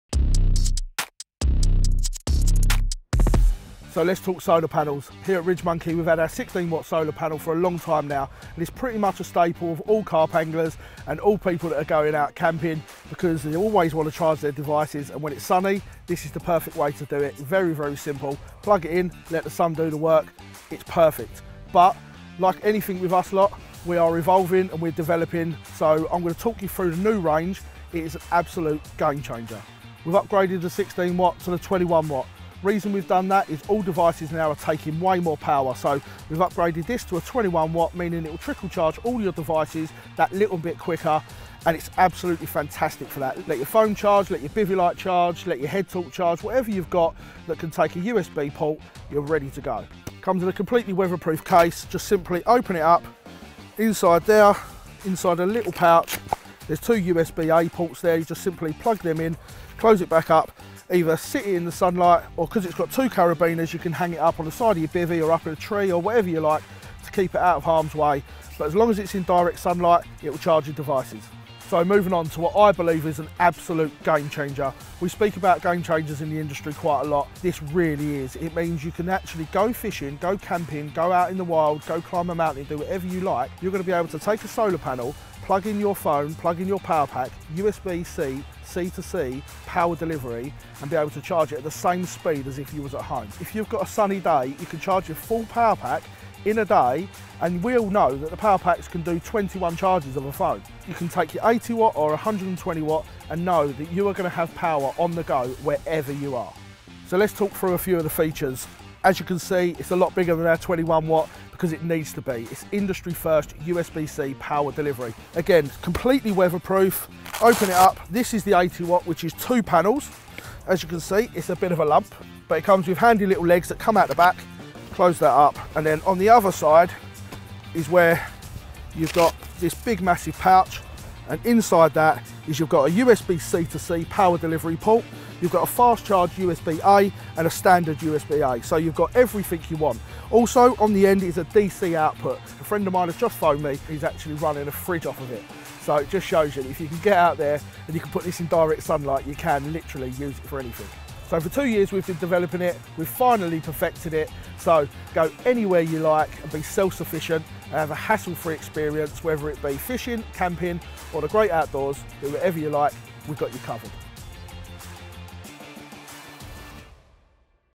RidgeMonkey Vault C-Smart Solar Panel napelemes töltők
RidgeMonkey Vault C-Smart PD 120W Solar Panel

A napelemes technológiának köszönhetően ingyenesen töltheti USB-eszközeit!

Az USB-A és USB-C portokkal, kompakt összecsukható kialakítással és masszív, időjárásálló külsővel a RidgeMonkey Vault C-Smart PD 120 W Solar Panel könnyen használható és szállítható, legyen szó túrázásról, kempingezésről vagy horgászatról.

Ezen kívül van egy elülső zsebe, hogy az összes vezetéket kéznél tartsa, és egy okos, merev hátsó szárny, amely lehetővé teszi a panel felállítását.

Ergonomikus kialakítású fogantyúval együtt szállítva.

Jellemzők:
- 1x 5V 2.4A USB-A kimenet
- 1x 18 W QC3.0 USB-A kimenet
- 1x 60 W USB-C tápellátási kimenet
- 1x 120W 20V DC kimenet
- Automatikusan felismeri a csatlakoztatott eszközt
- Automatikusan optimális áramot szolgáltat
- Időjárásálló külső
- Kompakt, összecsukható kivitel

Műszaki specifikáció:
- Súly: 4,4 kg
- Összecsukott méretek: 52 cm (sz) x 45 cm (hossz) x 5 cm (m)
- Nyitott méretek: 134 cm (sz) x 52 cm (h)